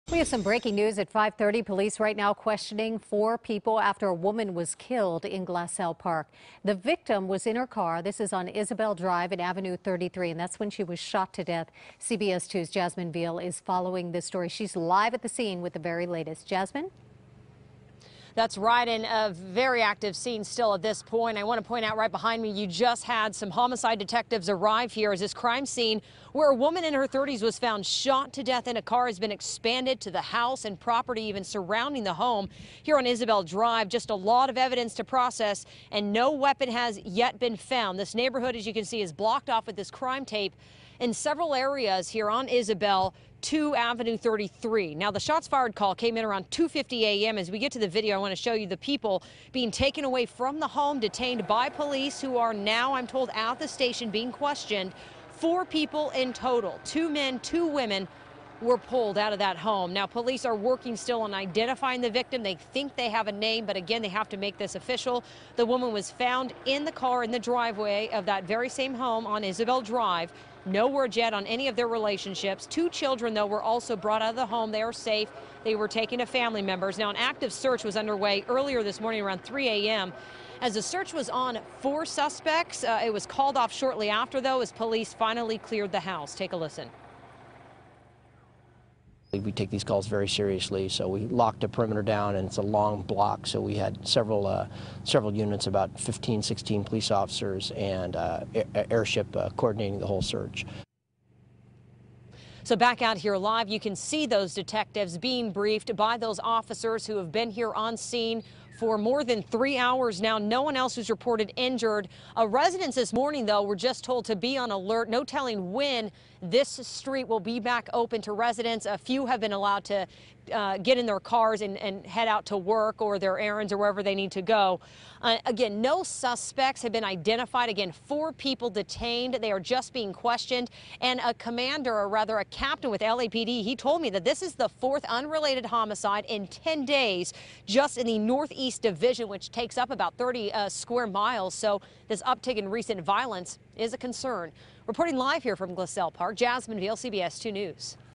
Woman Fatally Shot In Car Parked Outside Of Home In Glassell Park
A woman was shot and killed on Tuesday while she sat in a car that was parked in front of a home located in Glassell Park. CBS2's Jasmine Viel reports.